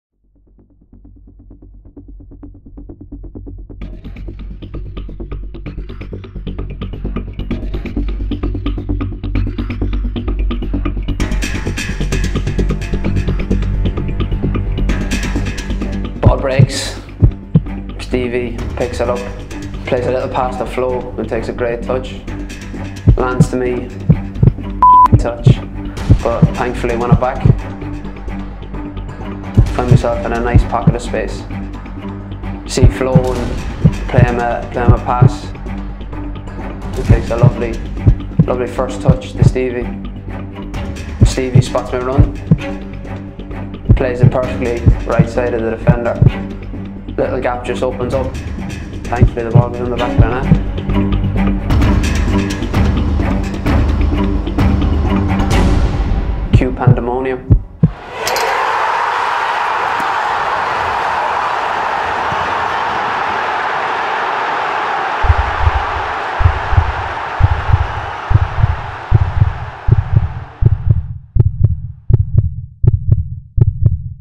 GOALS RETOLD | DARYL HORGAN
Daryl Horgan talks Hibs TV through THAT goal at Tynecastle ahead of Sunday’s Edinburgh Derby at Easter Road.